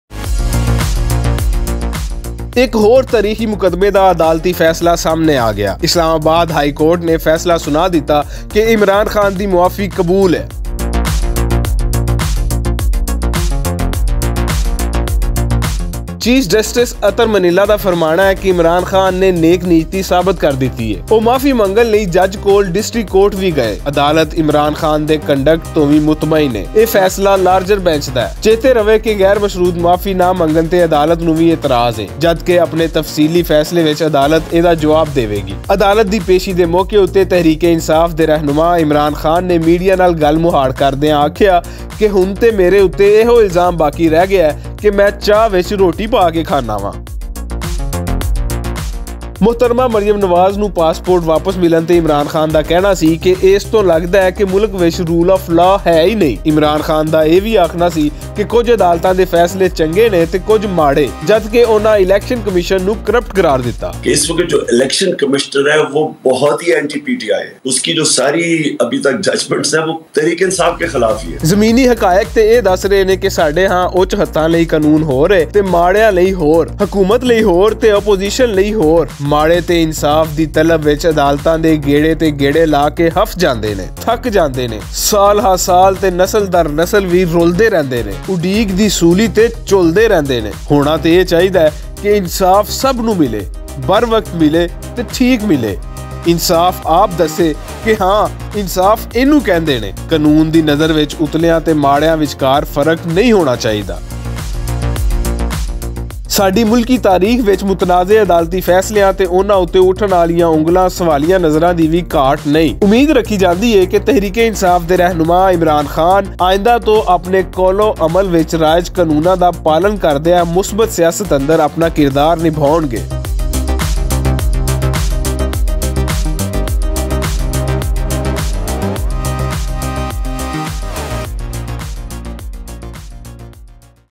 Imran Khan Statement After Relief From Islamabad High Court | Chaey mein Roti Daalna Mera Jurm Hai
اسلام آباد ہائیکورٹ توں ریلیف ملن توں بعد عمران خان دا بیان، چاہ وچ روٹی پا کے کھانا مای میرا جرم رہ گیا اے

punjabi language speaking,
punjabi language history,
punjabi language in pakistan,